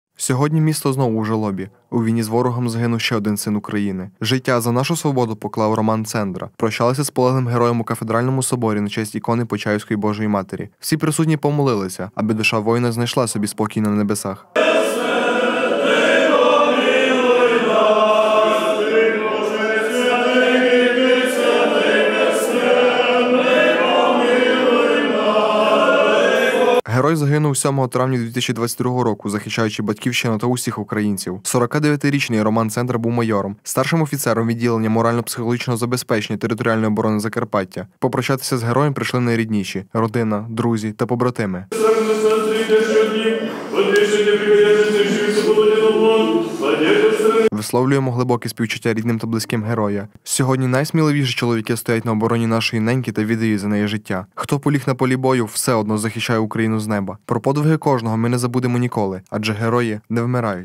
Захищає з неба. Місто знову у жалобі
Сьогодні місто знову у жалобі. у війні з ворогом загинув ще один син України. Життя за свободу поклав Роман Цендра. Прощалися з  полеглим Героєм у  Кафедральному соборі на честь ікони Почаївської Божої Матері.